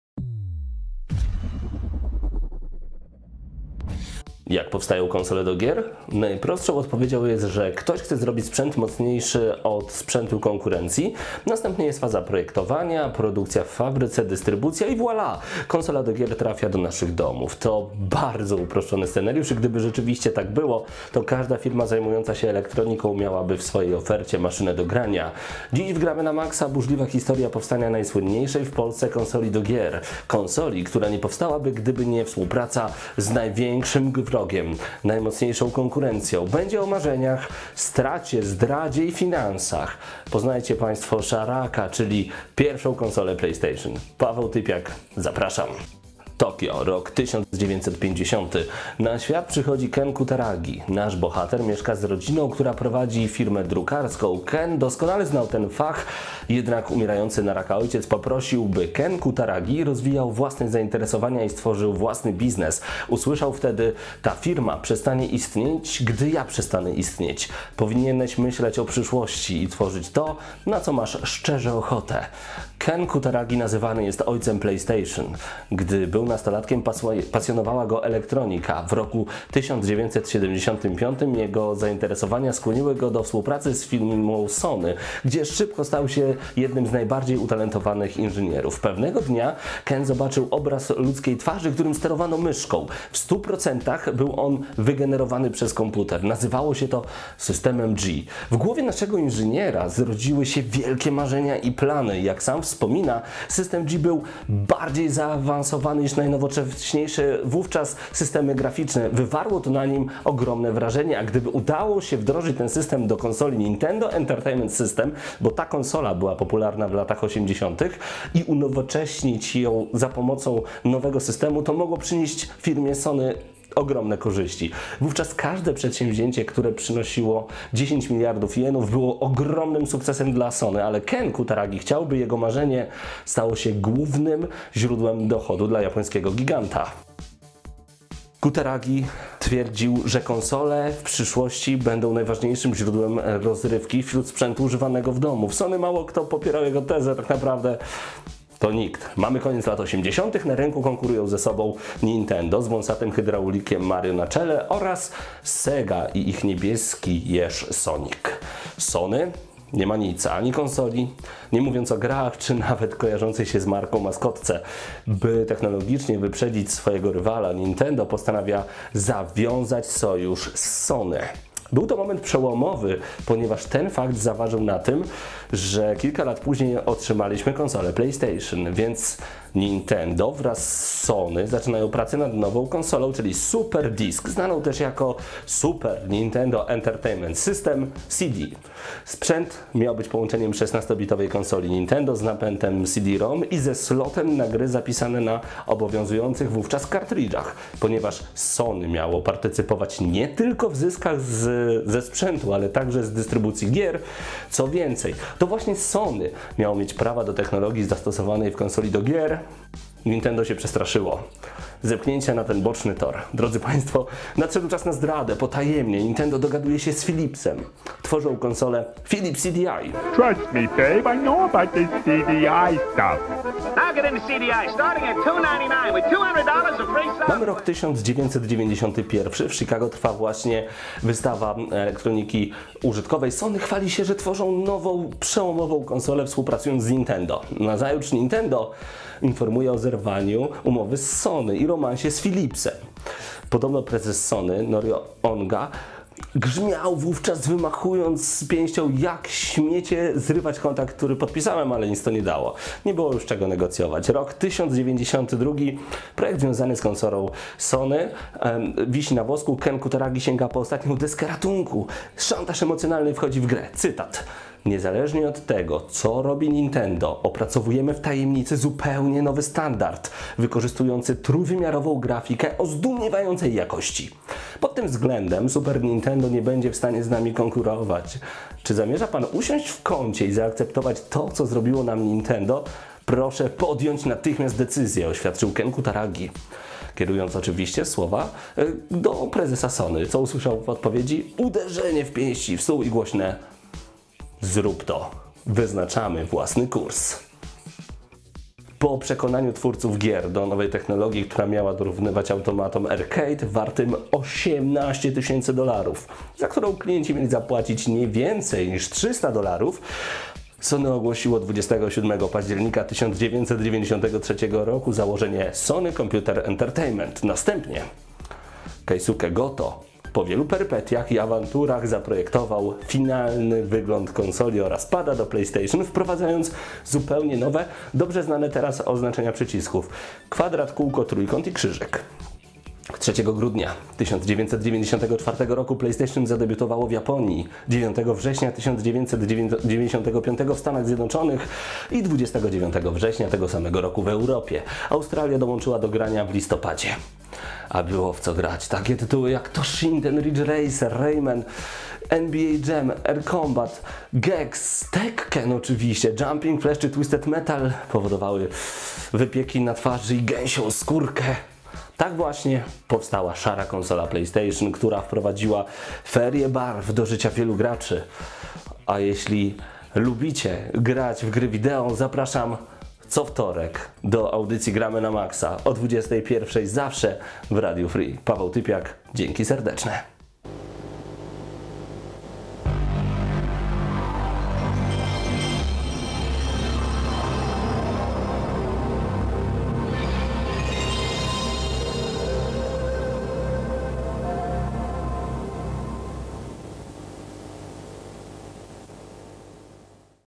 PlayStation - jak powstała pierwsza konsola Sony?
Paweł postanowił przypomnieć naprawdę interesującą i nietypową historię pierwszego PlayStation. Intrygującą, bo z... Nintendo w tle!